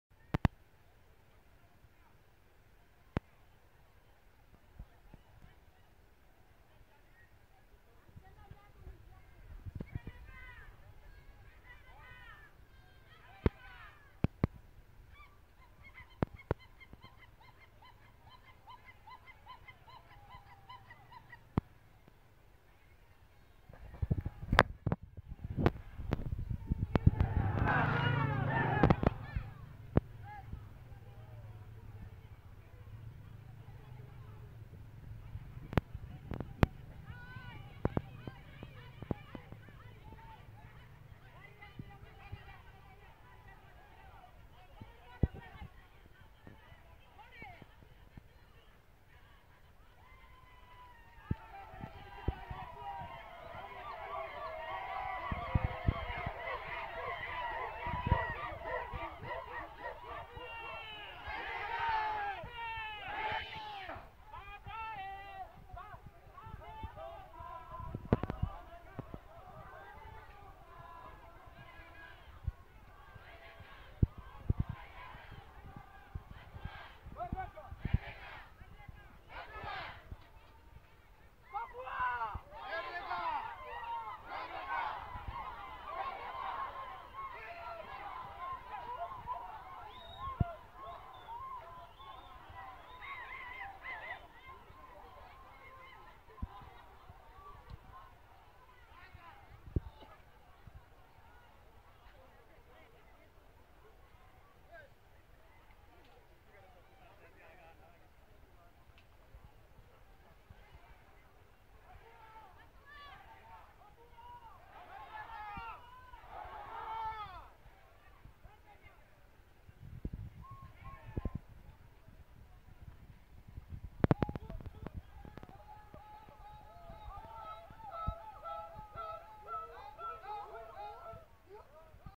Protes Tindakan Rasisme Masa Nabire menuju Kantor DPRD Nabire
Hidup MONYET